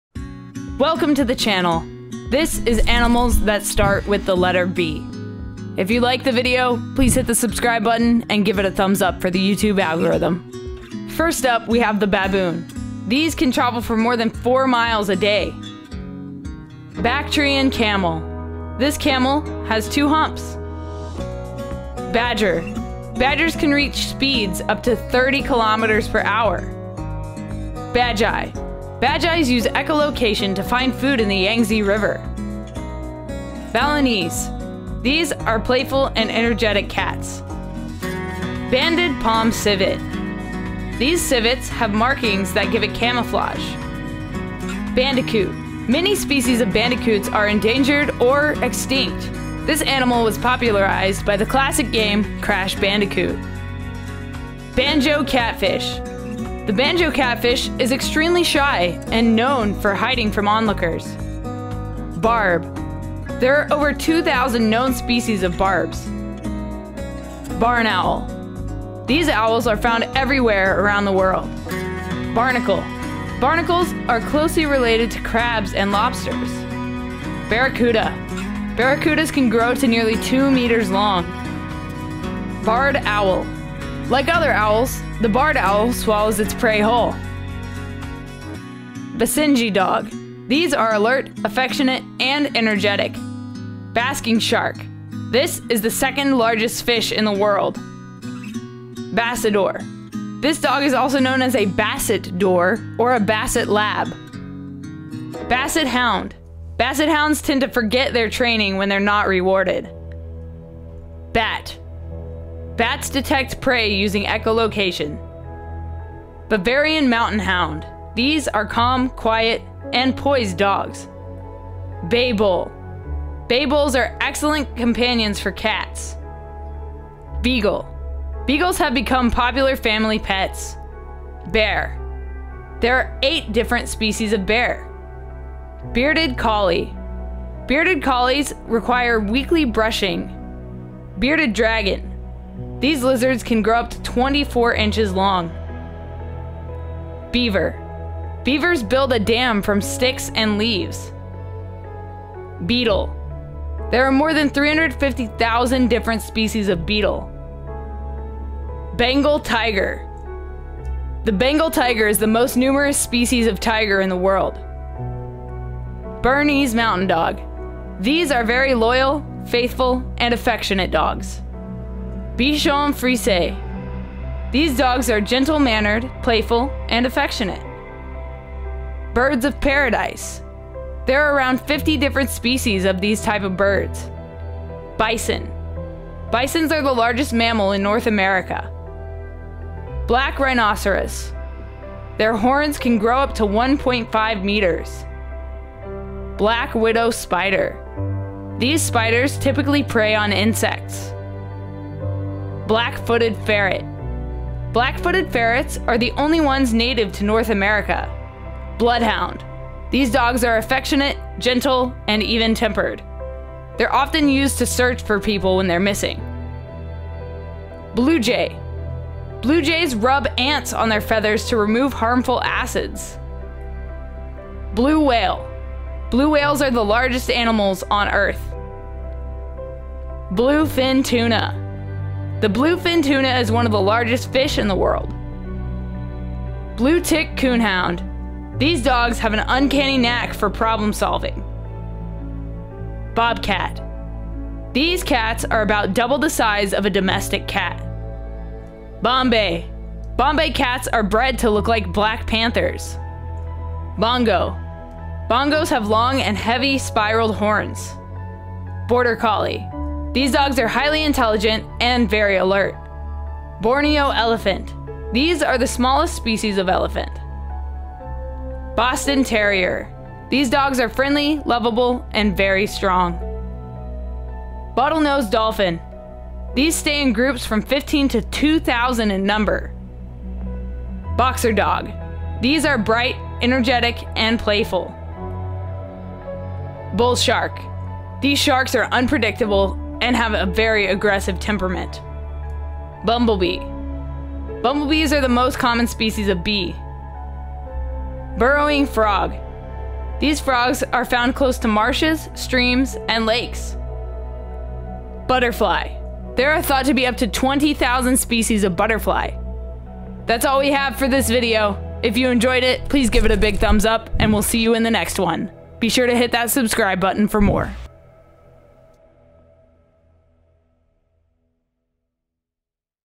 Animals that Start with The Letter B
A-Z- Animals tracks 112 different animals that start with the letter B, from baboon to butterfly fish. The most popular animal that starts with B is the Bengal Tiger, the least popular is the Burrowing Frog. Some fun facts about B name animals are:
There are thought to be as many as 20,000 different species of butterfly
The bonobo shares up to 97% of the same DNA as humans
The bison is the largest mammal in North America